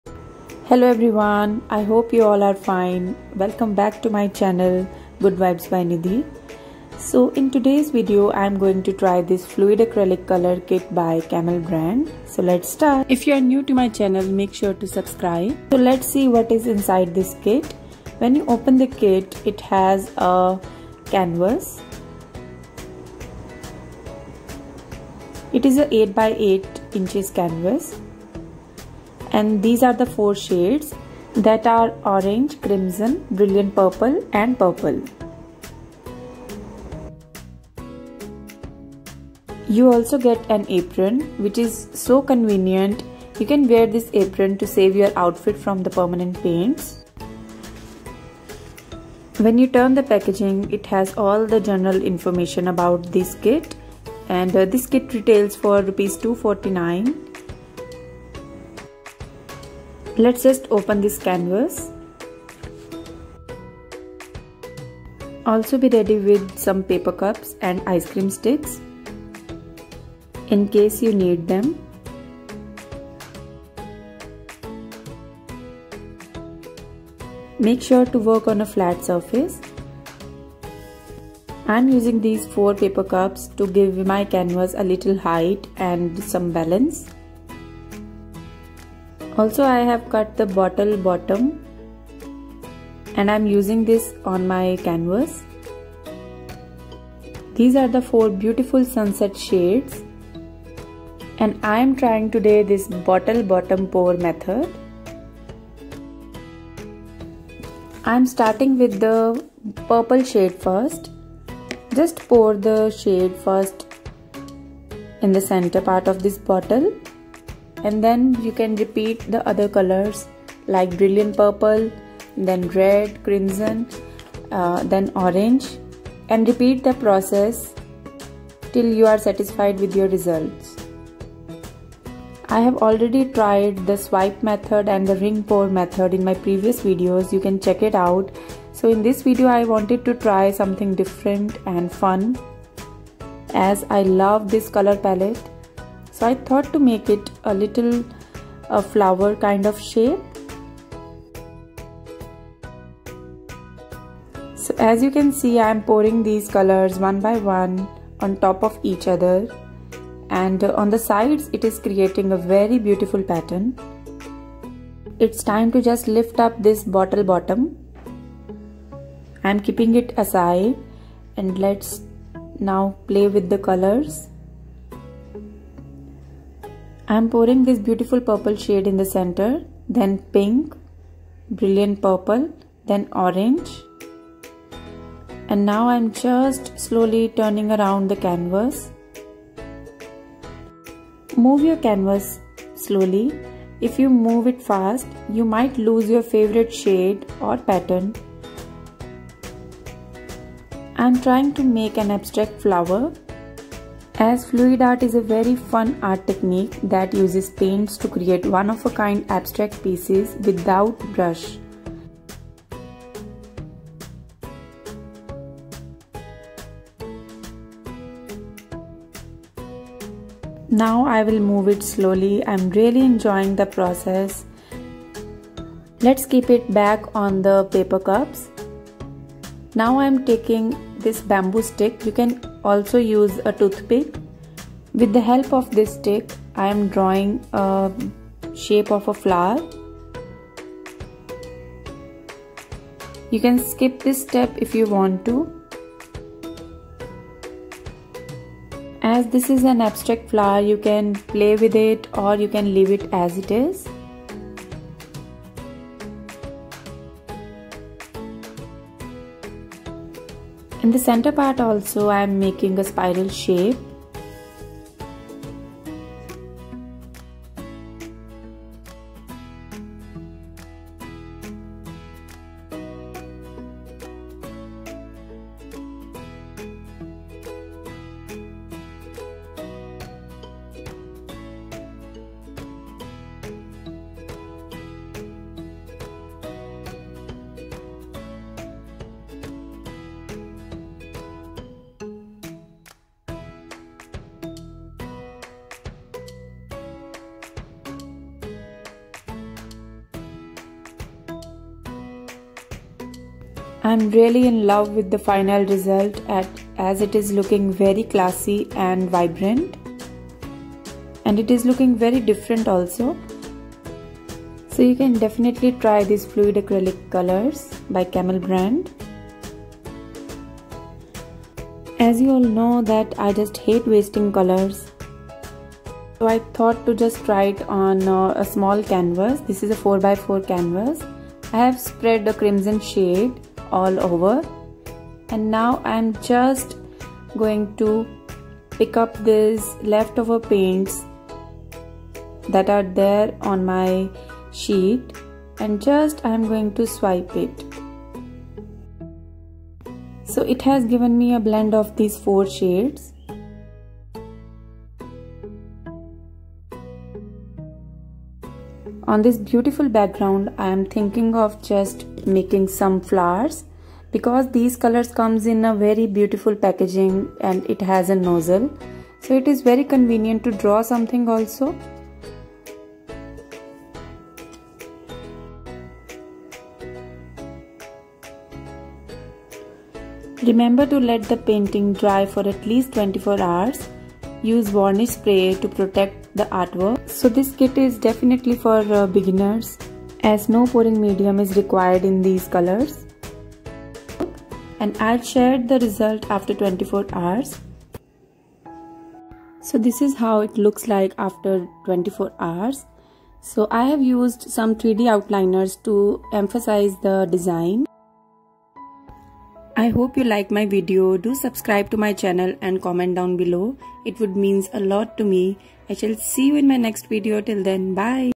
Camel Fluid Acrylic kit / Unboxing and Review/ 2 pouring techniques for beginners / fluid art
In today's video I am going to do Unboxing and reviewing the Fluid Acrylic Color kit by Camel .
I am trying two techniques here which you can easily try with this kit. This kit comes in 3 shades Sunset, Monochrome and Aqua Series.

If you recreate any of my paintings you can share with me on my Instagram:

I shall see you in my next video.
With Love and positive vibes,
Goodvibes by Nidhi.